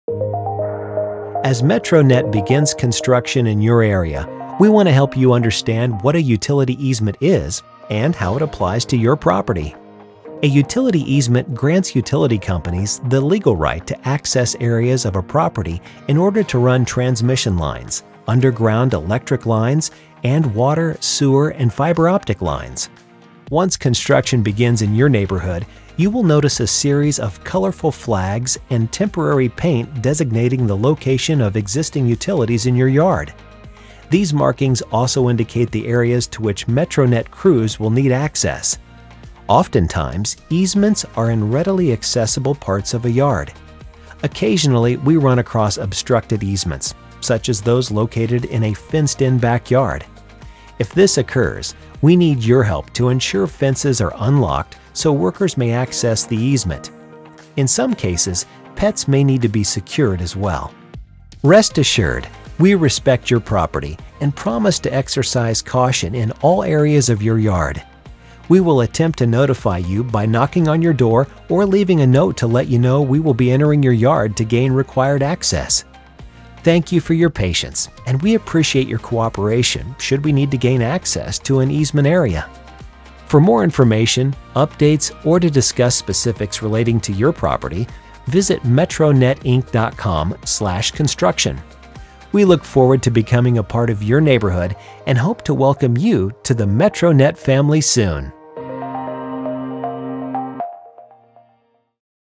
What is an easement?
This video explains what easements are and how MetroNet uses them to install their 100% fiber optic network.